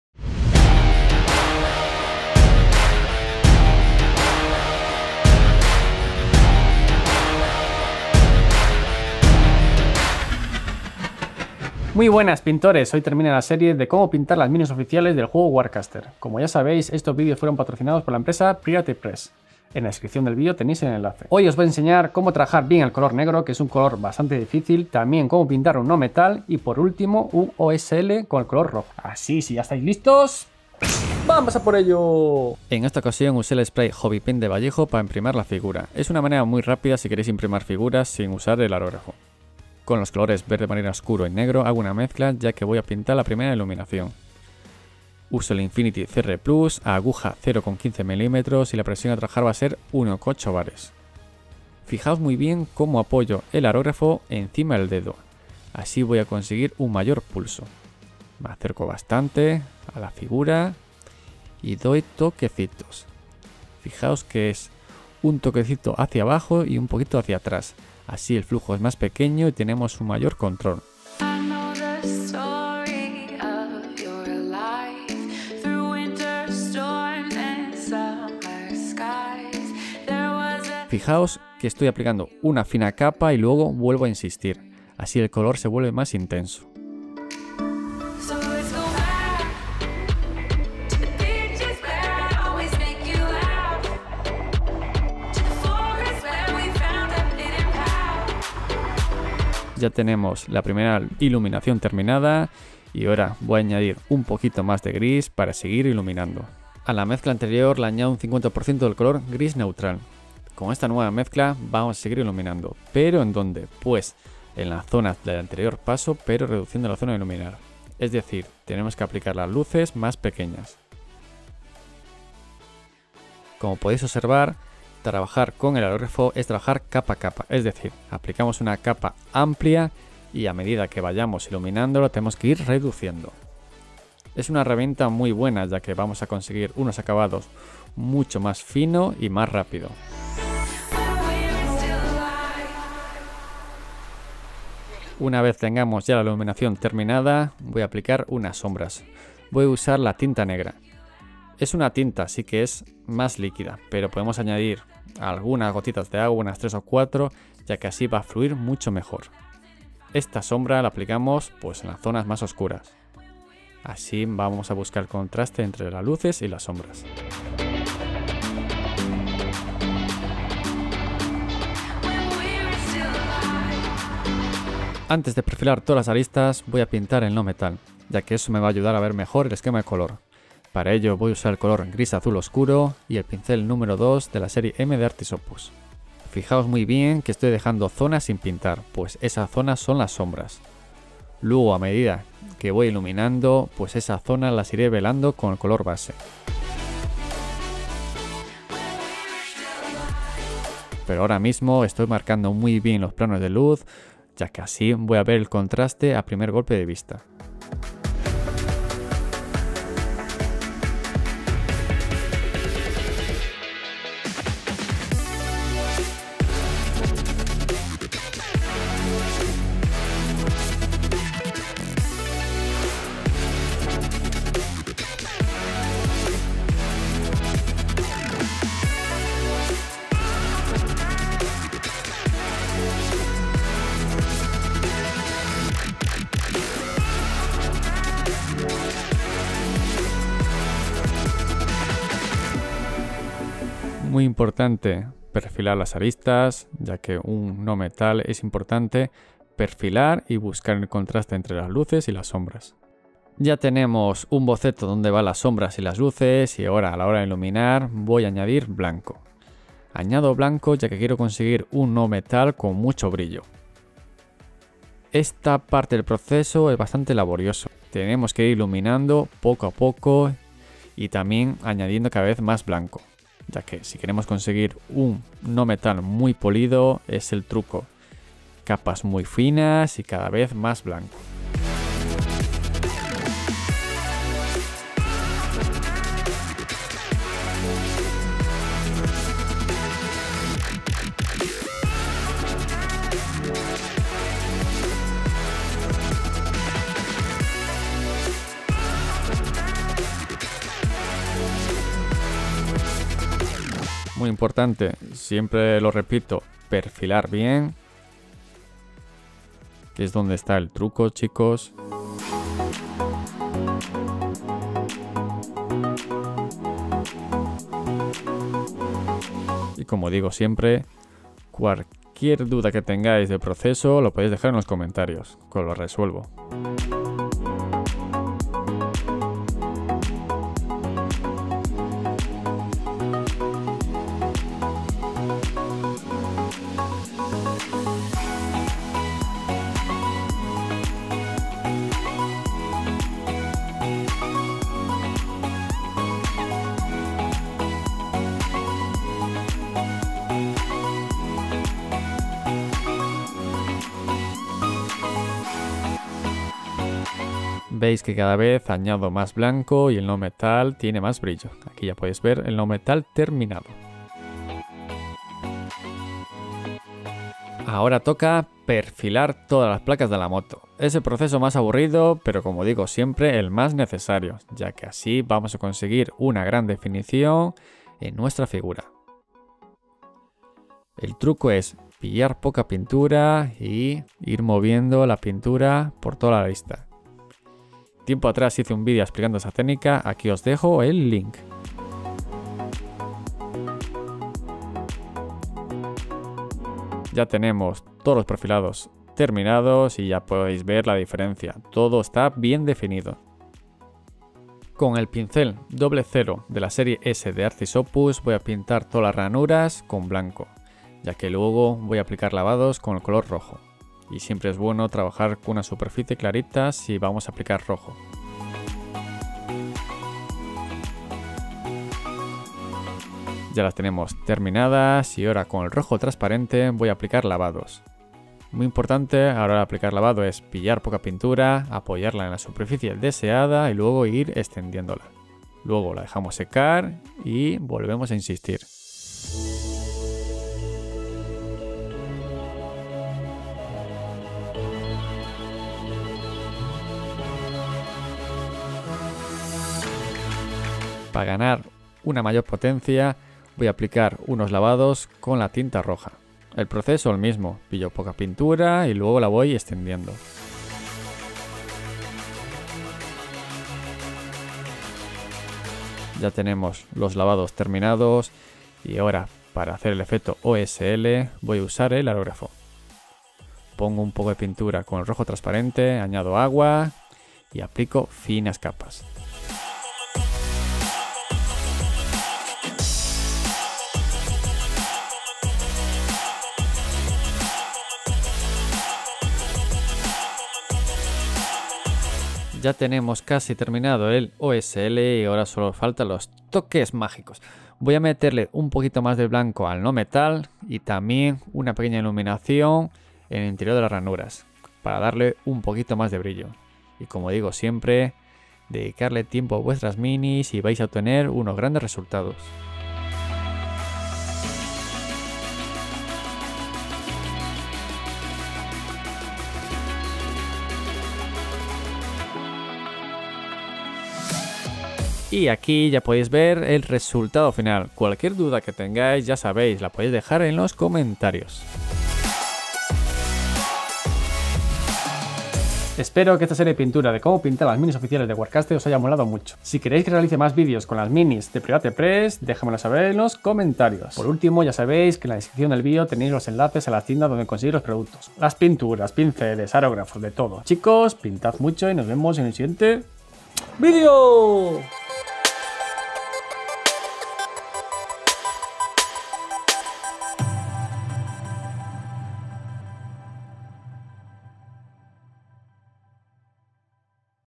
How to paint BLACK Armor ( airbrush + brush) / Como pintar una Armadura NEGRA ( Aerografo + pincel)
🤖WARCASTER MINIATURES 👇🏻

STORES WHERE YOU CAN BUY THE PRODUCTS
Racing Color ( la mejor tienda en aerografia)👇🏻